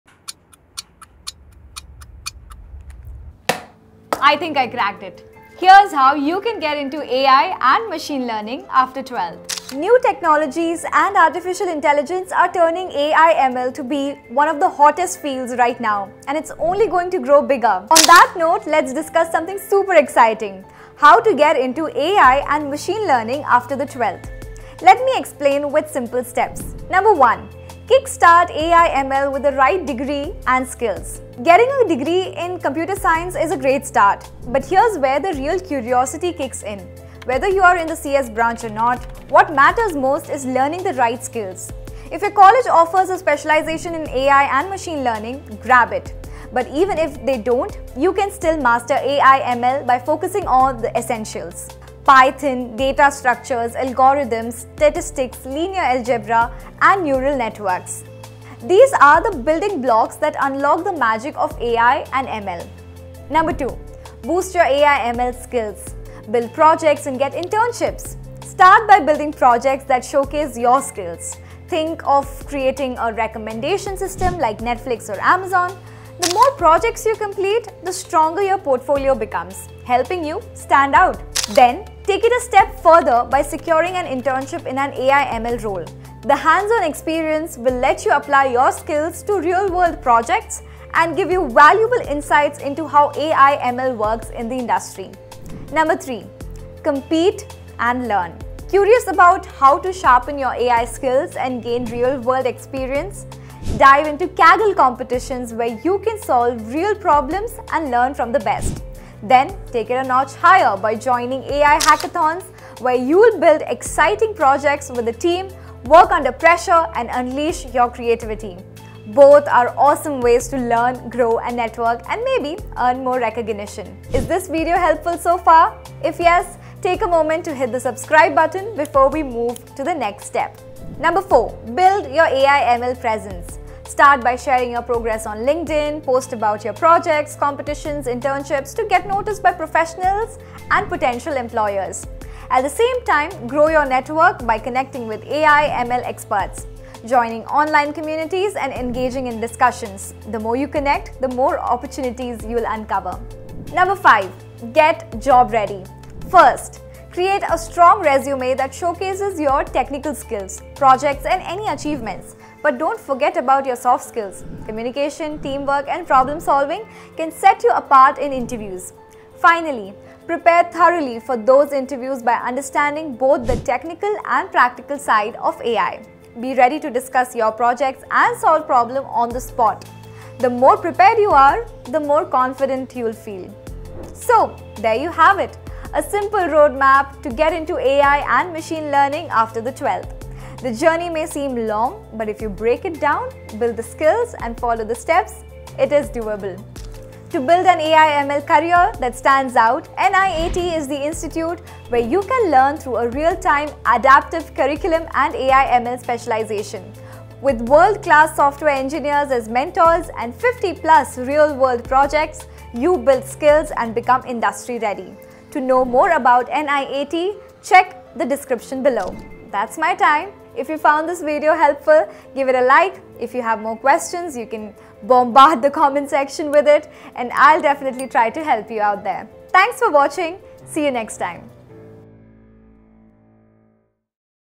The ONLY Machine Learning & AI Roadmap You NEED [Step-by-Step Guide] | NIAT
Curious about how to build a career in Artificial Intelligence and Machine Learning after finishing 12th? In this video, we’ll break down the essential steps to help you kickstart your journey in AI and ML, covering everything from the top courses and skills you need to real-world career opportunities.

Discover why AI and Machine Learning are the most in-demand fields today and learn how you can begin preparing yourself for a high-paying, future-proof career straight out of school.

🎓 Topics Covered:

- Key skills and courses for AI & ML
- Best education pathways and training
- Job roles and career opportunities
- Expert tips for success

Don’t wait—start building your tech career today! 🌐